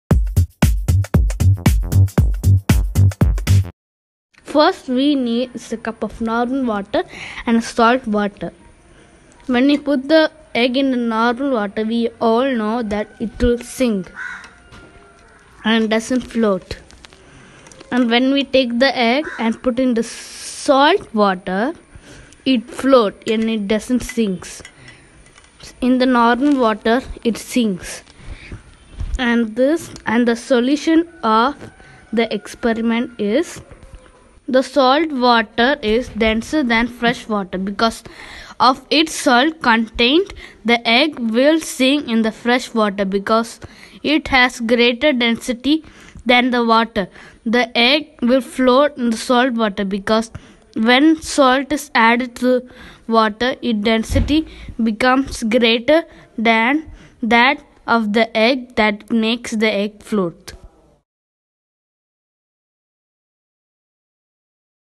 Science experiment PART 1
Science experiment for kids to show that an egg sinks in normal water but floats in salt water